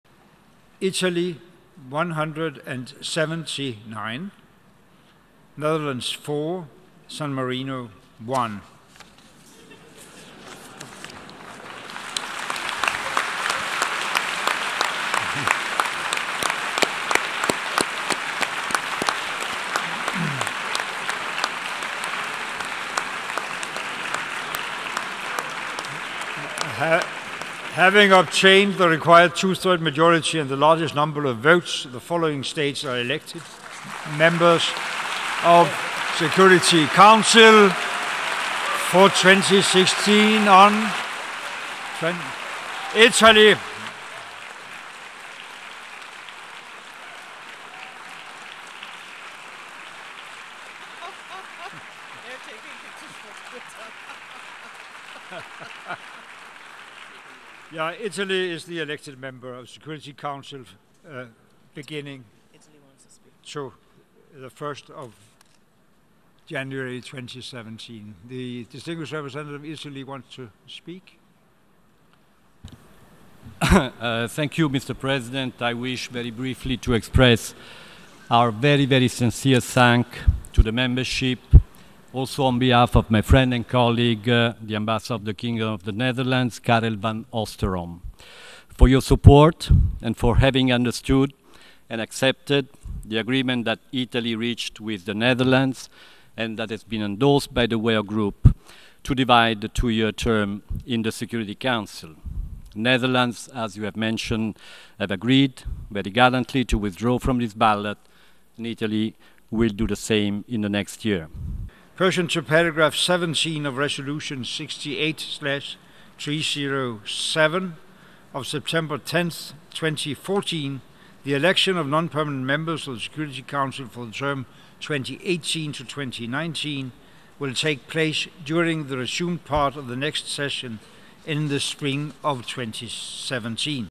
Italy to serve as UN Security Council non-permanent member
The UN General Assembly on Thursday elected Italy to a two-year term on the UN Security Council, joining Bolivia, Ethiopia, Kazakhstan and Sweden as the new non-permanent members from 2017, but with the understanding Rome will relinquish its seat at the end of 2017 and turn it over to the Netherlands for the remaining one-year period.